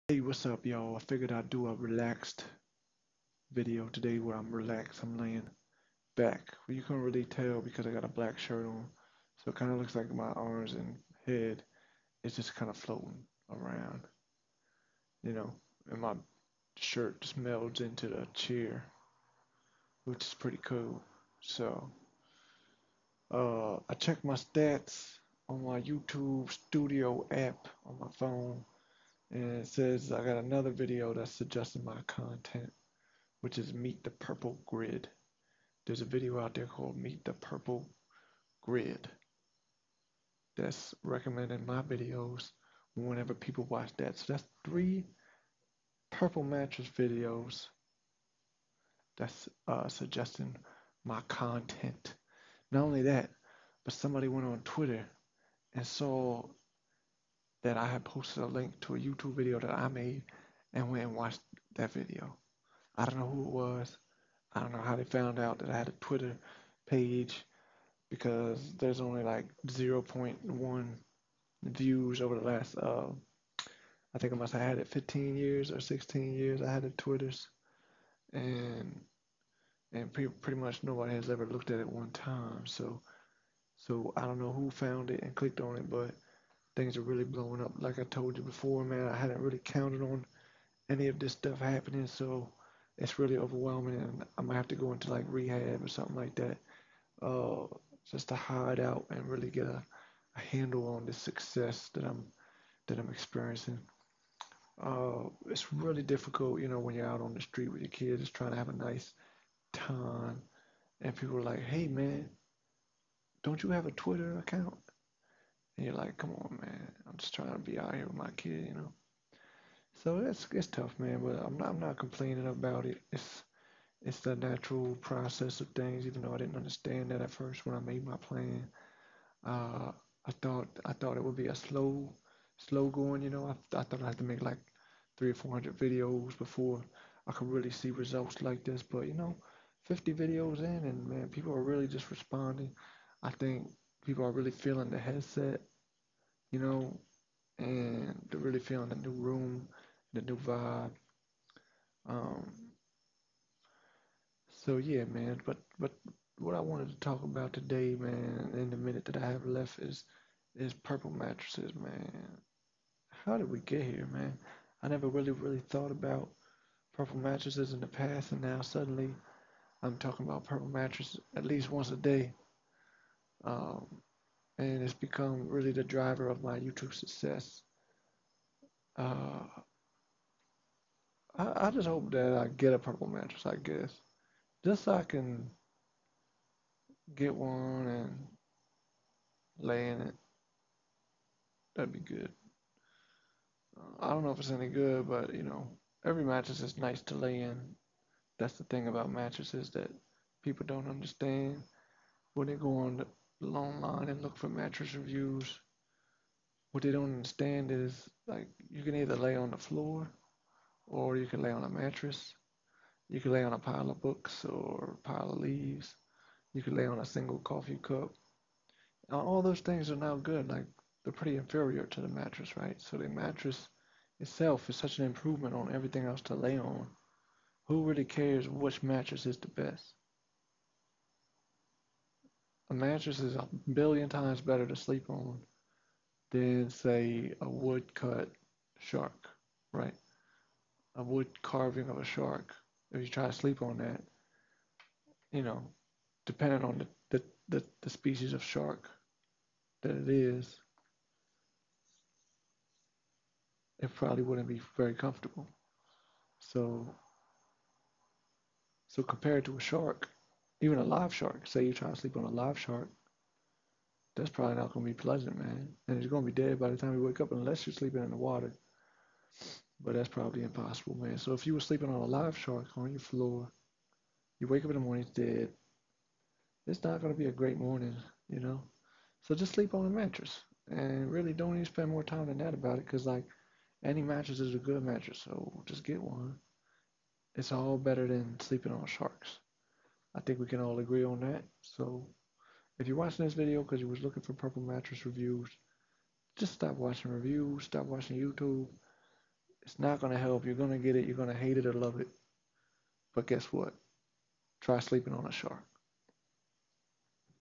Purple Mattress Grid Video is Suggesting My Videos? Give Me Some Space!
Stop watching mattress reviews on youtube y'all it's not going to help. And the fame from these algorithms is too much.